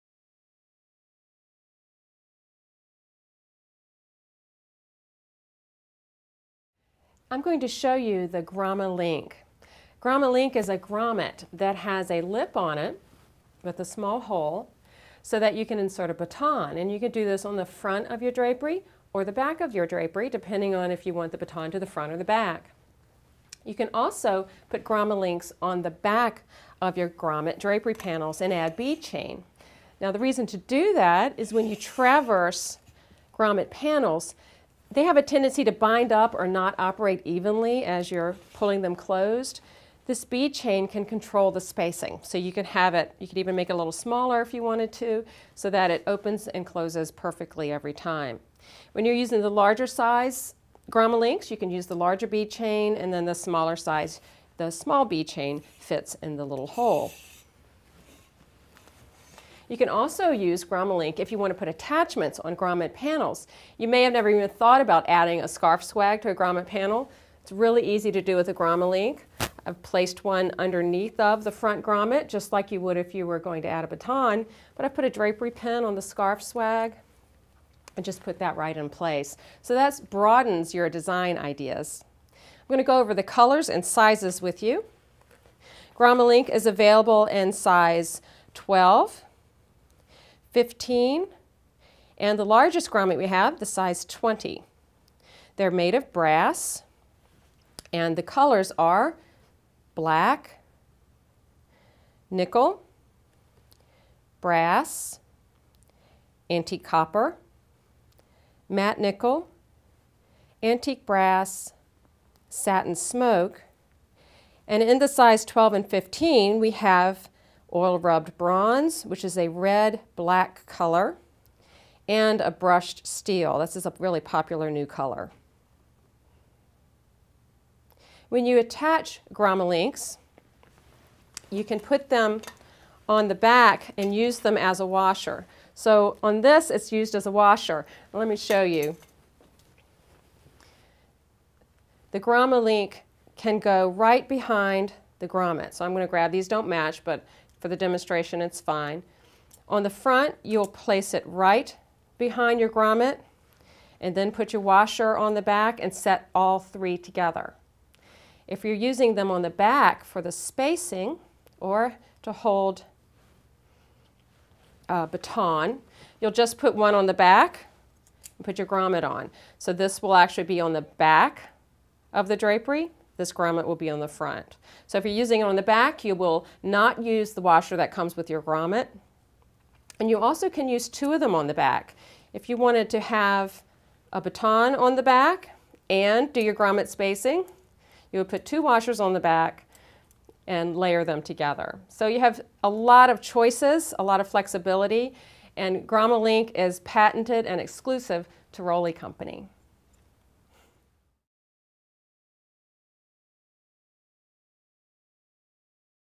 Grom-A-Link™ Grommets
See how to use Grom-A-Link™ on your grommet drapery panels to add batons, swag and banner embellishments or to control spacing.